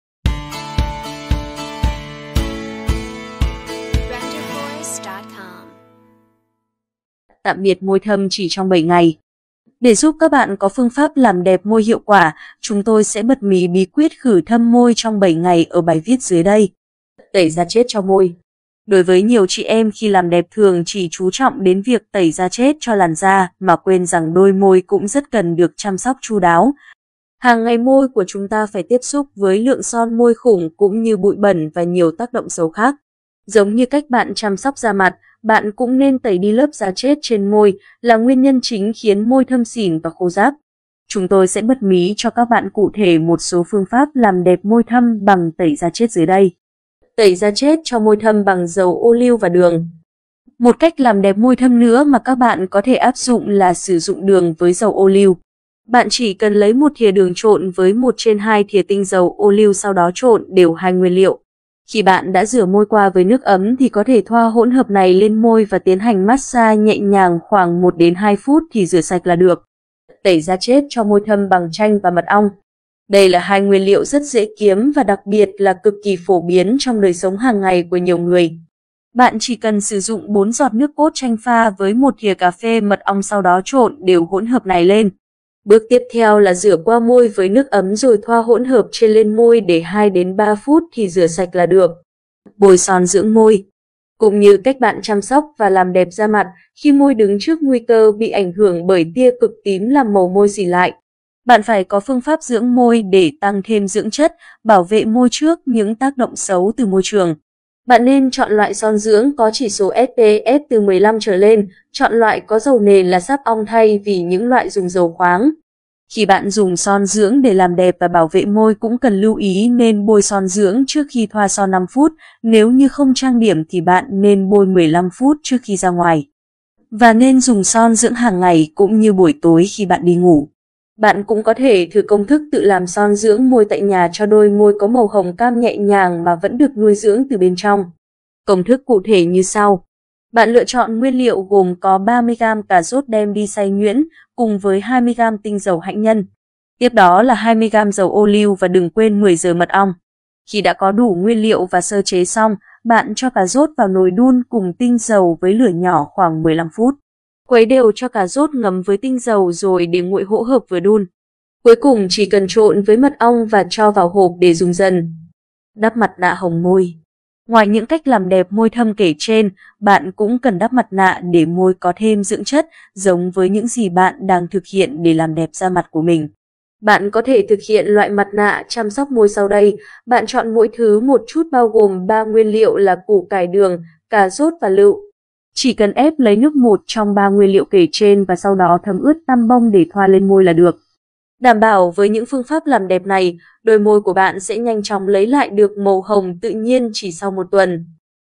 Tạm biệt môi thâm chỉ trong 7 ngày
Tạm biệt môi thâm chỉ trong 7 ngày Để giúp các bạn có phương pháp làm đẹp môi hiệu quả, chúng tôi sẽ bật mí bí quyết khử thâm môi trong 7 ngày ở bài viết dưới đây. Đối với nhiều chị em khi làm đẹp thường chỉ chú trọng đến việc tẩy da chết cho làn da mà